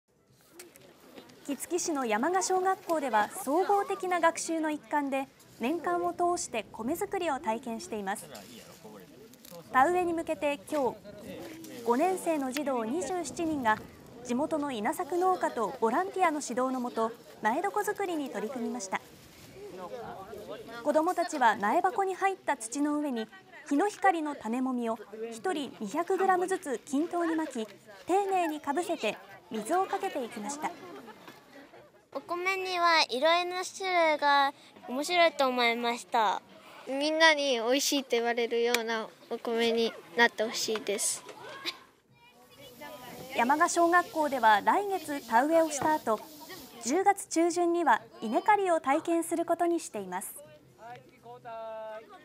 学校で児童が苗床づくり 稲の種まきを体験して米作りを学ぶ
大分県杵築市の小学校で米作りを学ぼうと、子どもたちが苗床づくりに取り組み、種まきを体験しました。